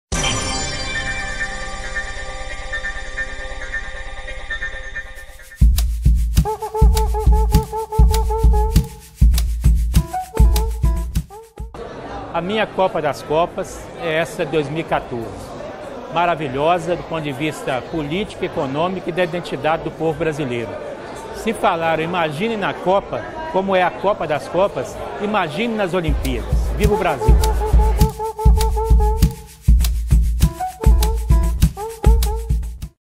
MinhaCopaDasCopas | André Xavier
Depoimento para o projeto MinhaCopaDasCopas da Fundação Perseu Abramo